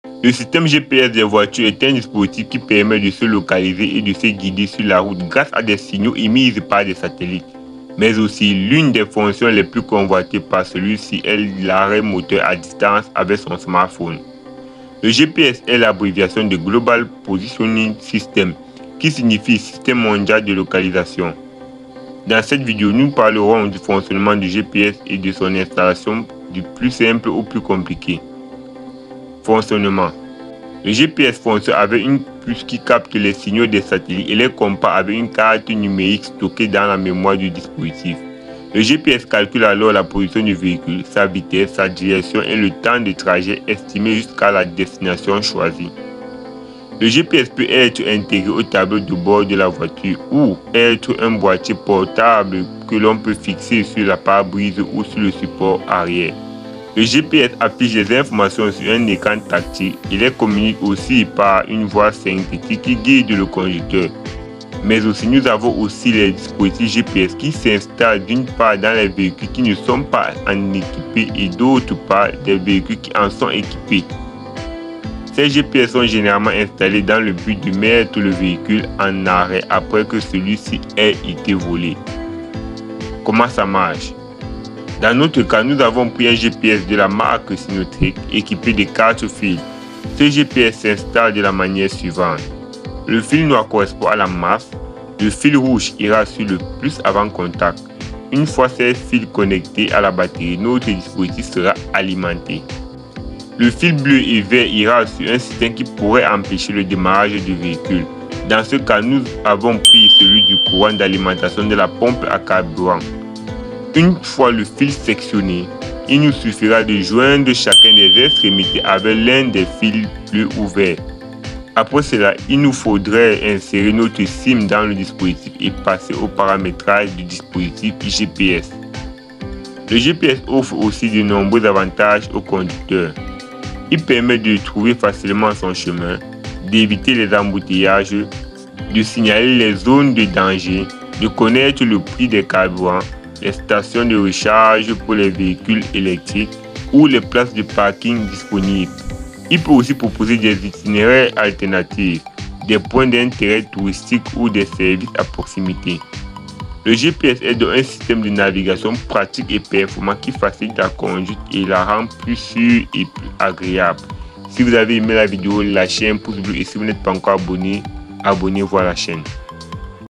GPS voiture : Fonctionnement et schéma d'installation d'un gps sur une voiture
Le système GPS des voitures est un dispositif qui permet de se localiser et de se guider sur la route grâce à des signaux émis par des satellites, mes aussi, l'une des fonctions les plus convoité est celui d'arrêt moteur a distance avec son smartphone. Le GPS est l'abréviation de Global Positioning System, qui signifie système mondial de localisation. dans cette vidéo nous parlerons du fonctionnement du gps et
de son installation du plus simple au plus compliqué

PDF d'installation GPS disponible

🤓Si tu est intéressé pas une formation en ligne écrit moi en IB👉👇👇 ou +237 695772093